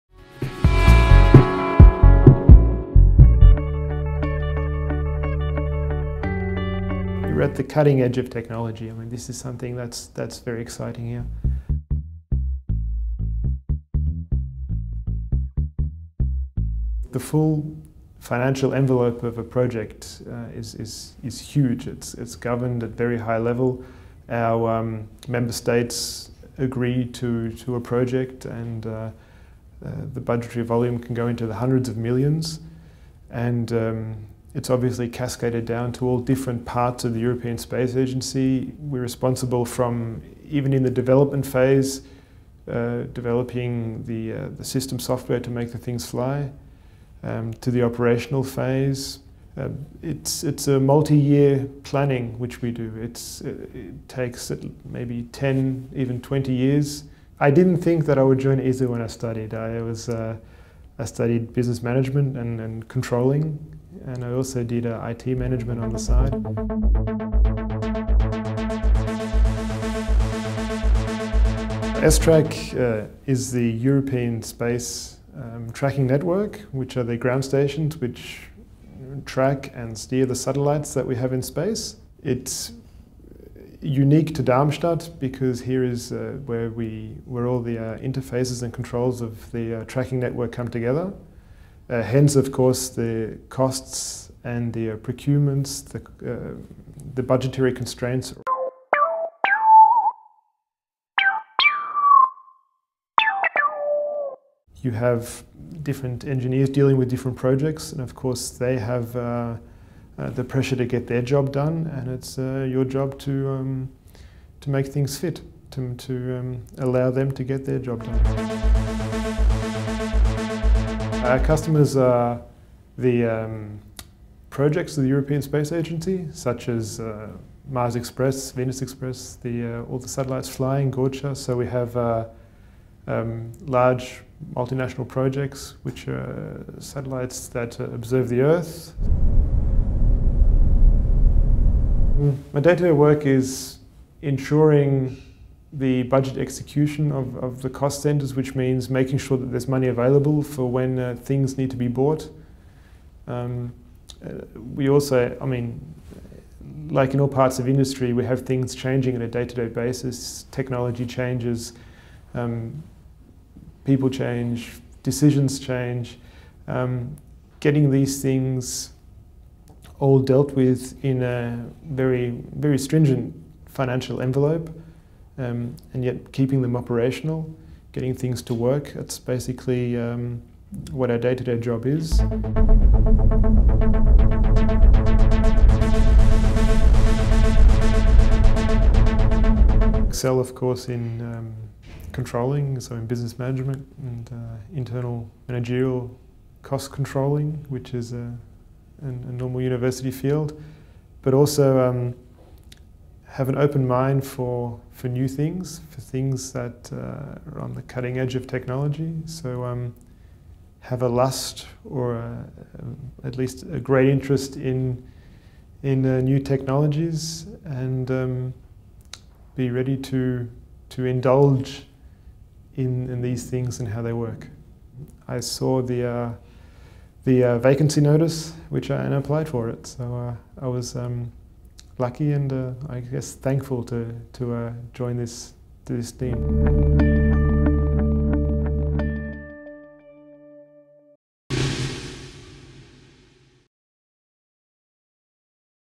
Sven Schweinfurth supervises space mission budgets
Sven Schweinfurth is a Resource Management Officer at ESA. In this video he talks about the various criteria involved in cost controlling and ensuring budget execution in a multinational agency. Sven also discusses how the ESTRACK European space tracking network is used and how keeping up with changes in technology is a crucial part of his job.